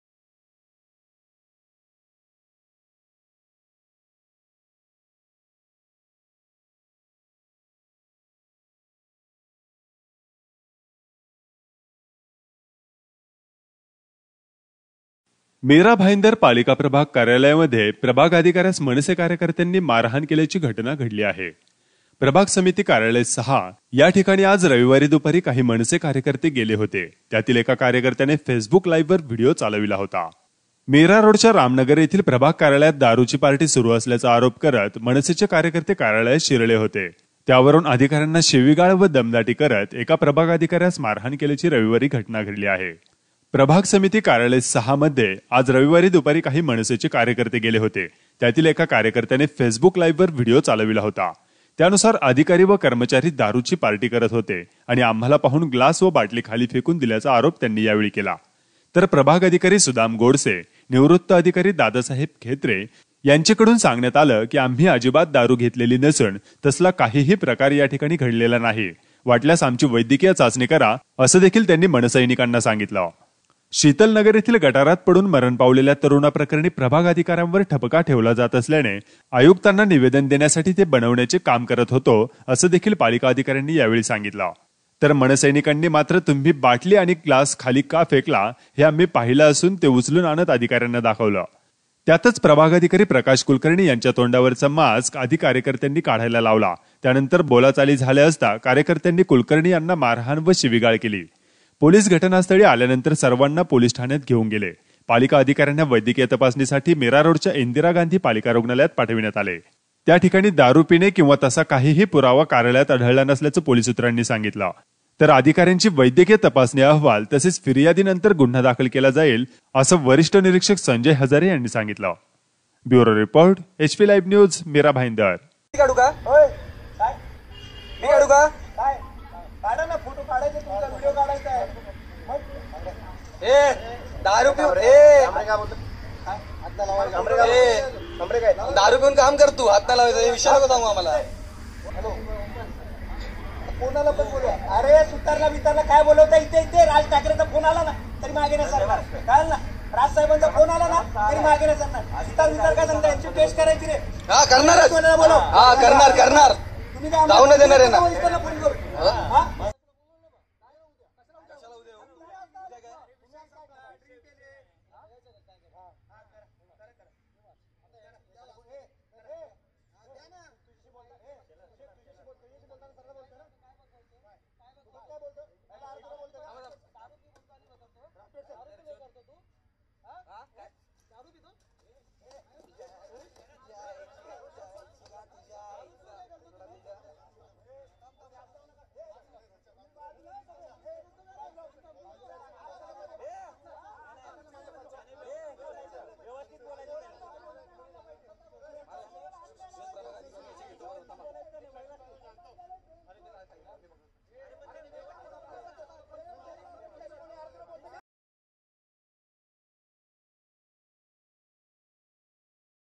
MNS workers' assault MBMC ward officer
मीरारोडच्या राम नगर प्रभाग समिती कार्यालयात मनसे कार्यकर्त्यांनी महापालिका प्रभाग अधिकारी प्रकाश कुलकर्णी याना केली मारहाण